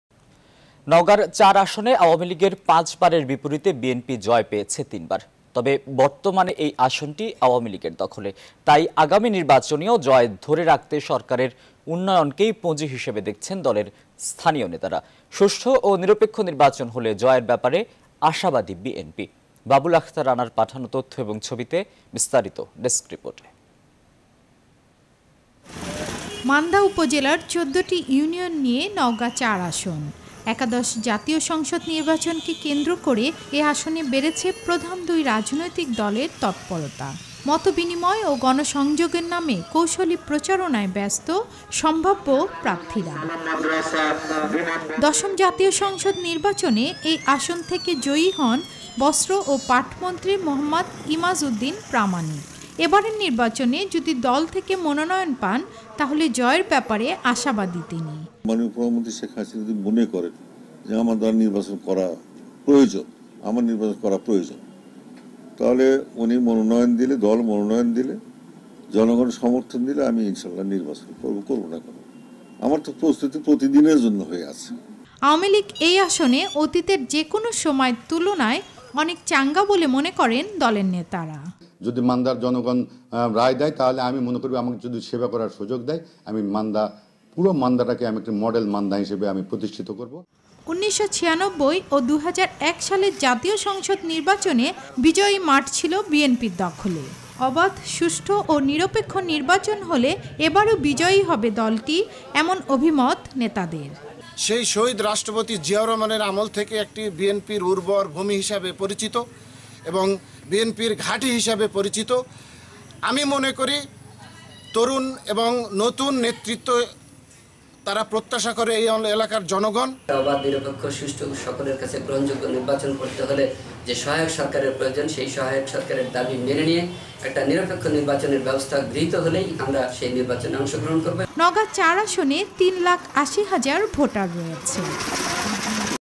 Election 2018/নওগাঁর ৪/আসনটি আওয়ামী লীগের দখলে/এবার জয়ের ব্যাপারে আশাবাদী বিএনপি 25Oct.18
নওগাঁর চার আসনে আওয়ামী লীগের পাঁচবারের বিপরীতে বিএনপি জয় পেয়েছে  তিনবার।  তবে বর্তমানে এ  আসনটি  আওয়ামী লীগের দখলে। তাই আগামী নির্বাচনেও জয় ধরে রাখতে সরকারের উন্নয়নকেই পুঁজি হিসেবে দেখছেন দলের স্থানীয় নেতারা। সুষ্ঠ ও নিরপেক্ষ নির্বাচন হলে জয়ের ব্যাপারে আশাবাদী  বিএনপি। বাবুল আখতার রানার তথ্য ও চিত্রে ডেস্ক রিপোর্ট ।

Our other YouTube channels:
News24 Official Address:
NEWS24
371/A
Bashundhara Road
Block – D
Bashundhara Residential Area
Dhaka - 1229